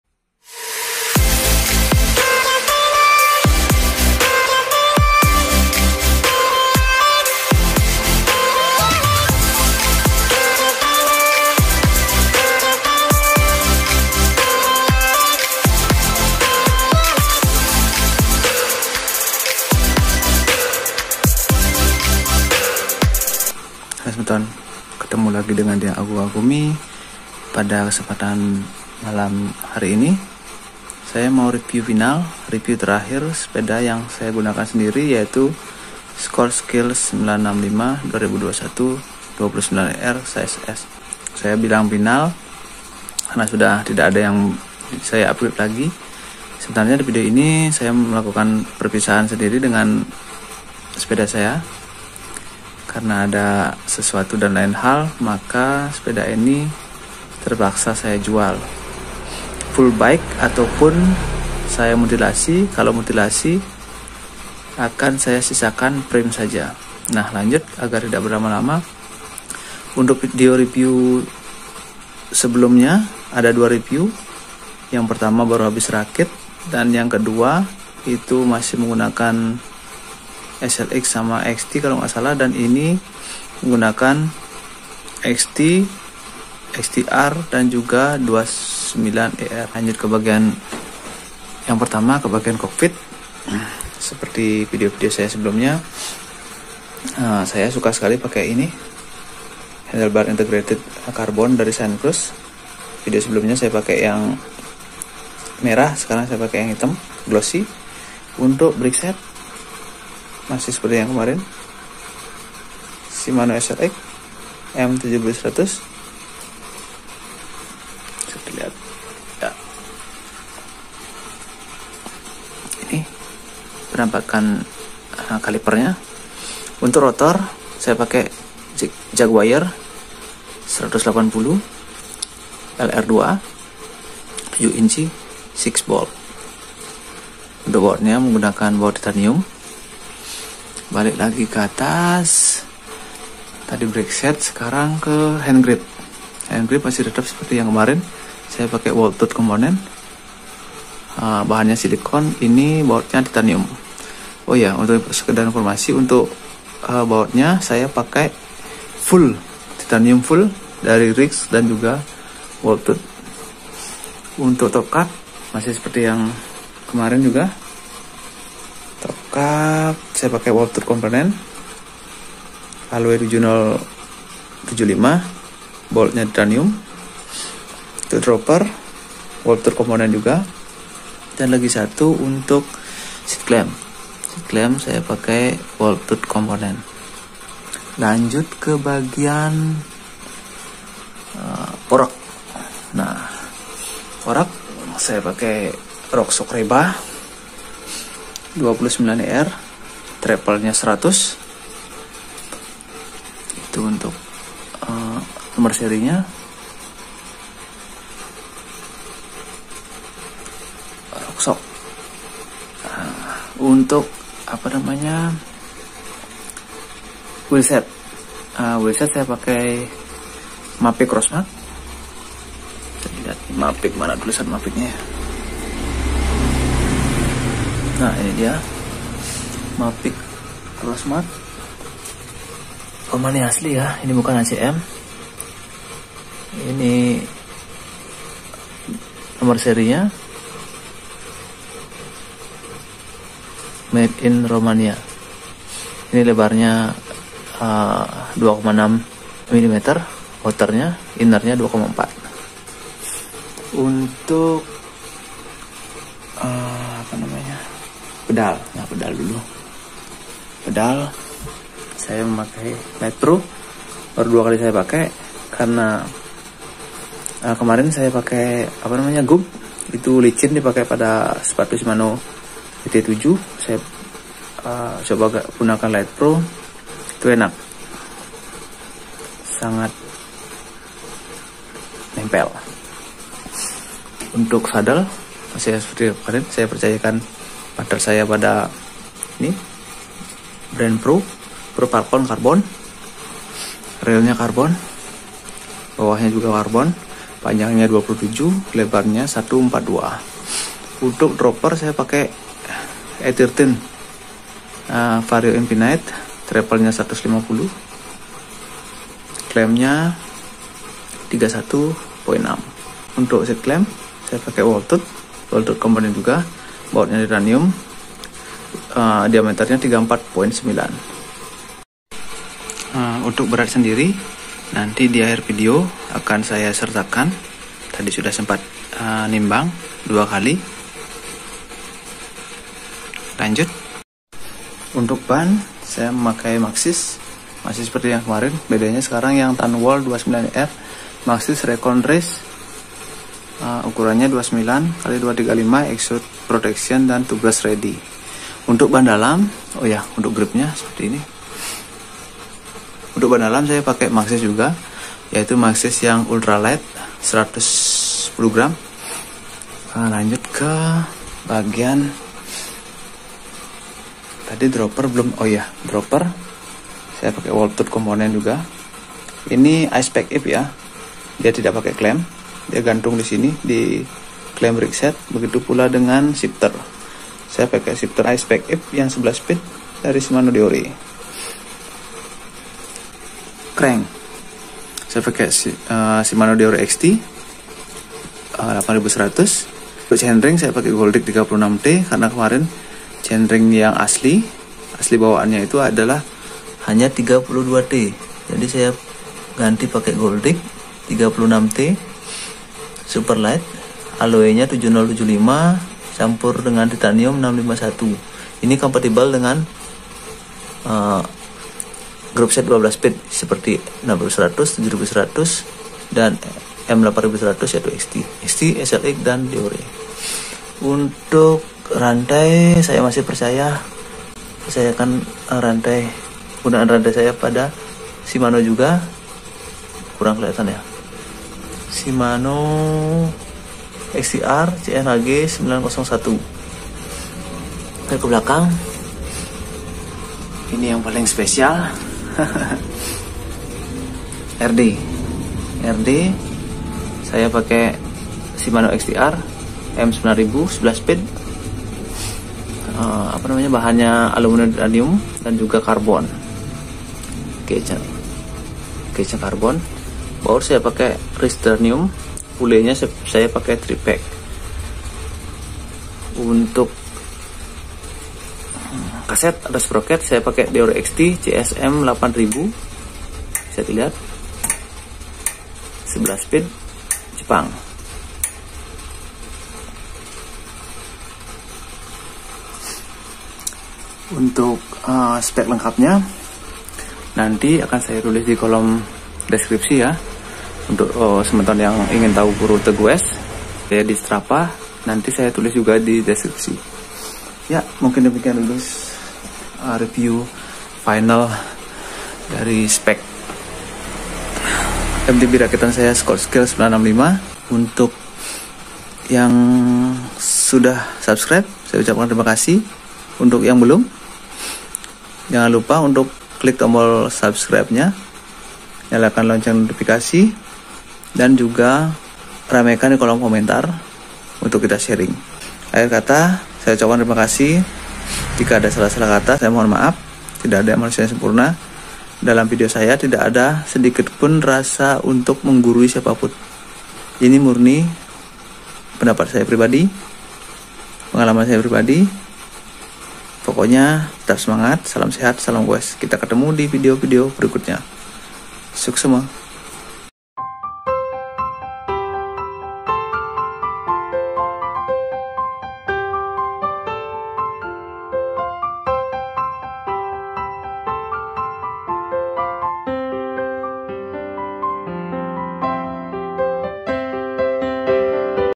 Sepeda MTB Rakitan Scott Scale 965 || Spek Jos banget !!
Sepeda MTB Rakitan Scott scale 965 29er size
tahun 2021 spek mantap..

Spek lengkap :

- Frame scott scale 965 29er S 2021
- Fork Rockshox Reba RL T100 29er Air
- Wheelset Mavic Crossmax PR Boost TA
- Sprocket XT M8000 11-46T
- RD XTR M9000
- Shifter I-SpecEV 11Sp
- Chain ShimanoXTR CN HG901
- Crankset XT m8100 ( Chainring Goldix 36T)
- Brakeset SLX M7100 Dual piston
- Dropper seatpost E*tirteen 31.6 x 400mm
- Remote dropper wolftooth
- Rotor Jagwire PRO LR2 180mm
- Ban Maxxis Rekon Race 29 x 2.35
- Saddle Carbon Pro Falcon Anatomic Fit 142
- Pulley Tripeak
- BB XT M800
- Headset FSA orbid
- Baut2 full titanium
- Seatclamp, Top cap, Bar end, Handgrip merk Wolftooth

bobot keseluruhan : 10.9kg

Beberapa akun sosmed saya
Strava dan FB : Agumy Krisnanda

semoga informasi ini bermanfaat, salam sehat salam gowes